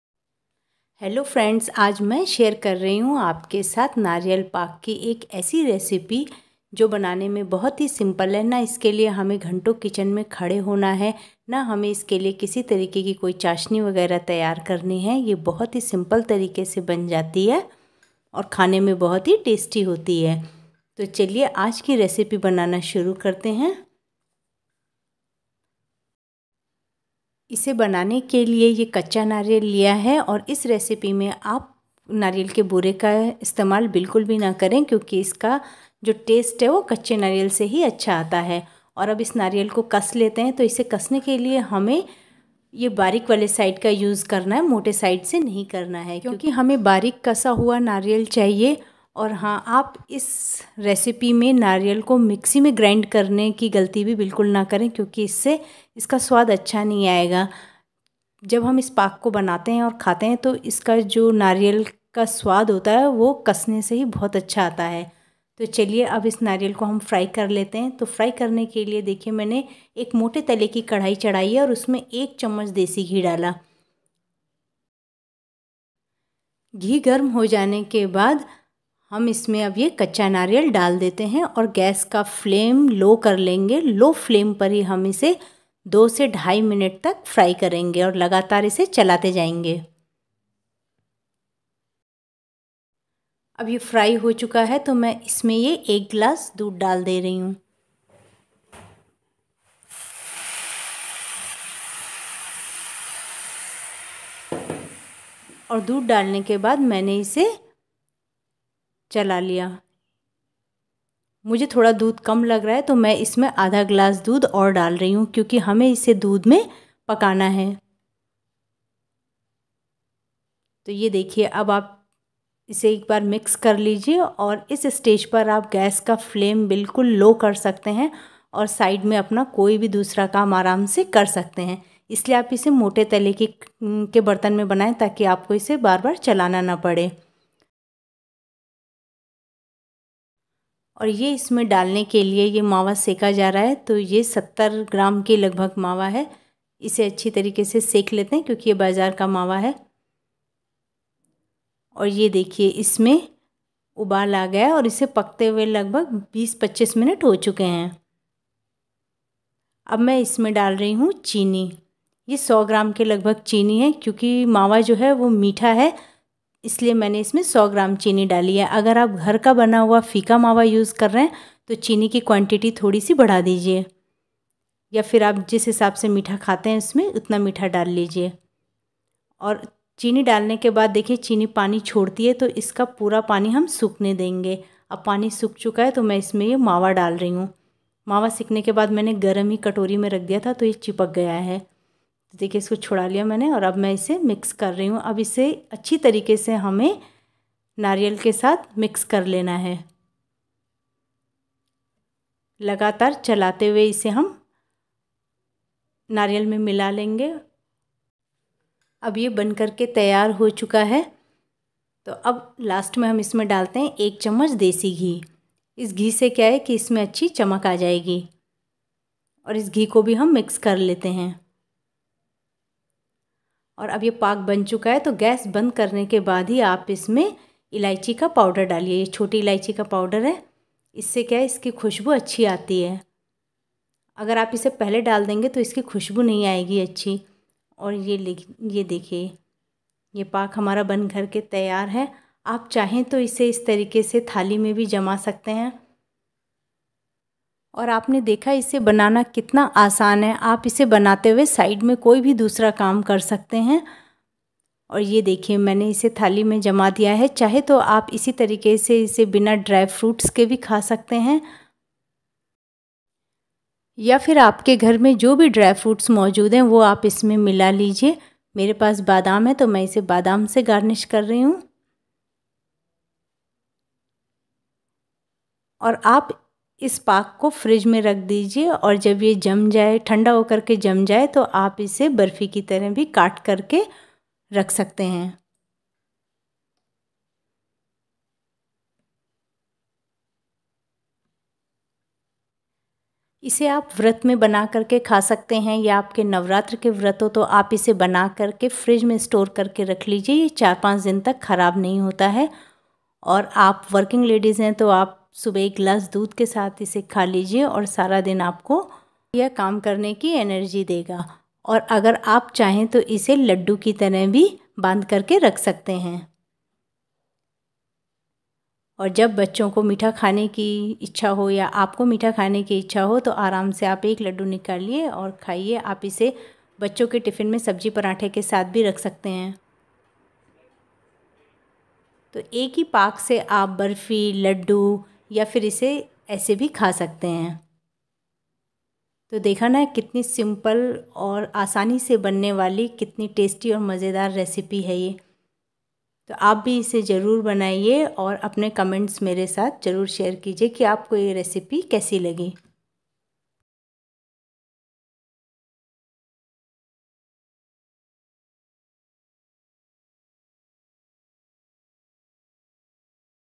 कम मेहनत मे, नारियल पाक, आसान तरीके से बनाए, nariyal Pak
कम समय,कम मेहनत और आसान तरीके से बनने वाली मिठाई ।